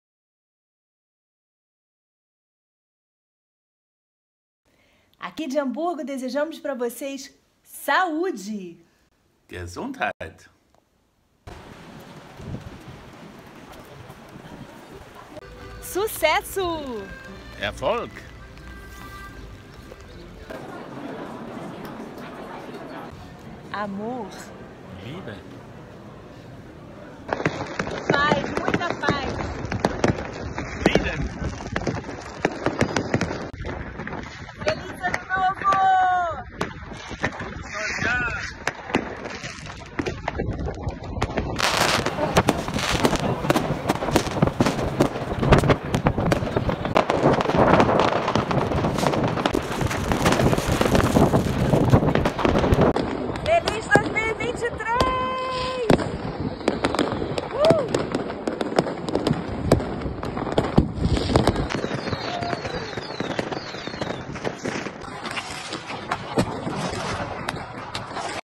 FELIZ ANO NOVO DE HAMBURGO, ALEMANHA
O Ano de 2022 chegou ao fim. Neste vídeo, desejamos (eu e meu companheiro) um excelente 2023 para vocês.
Obrigada por acompanharem o canal. Até breve!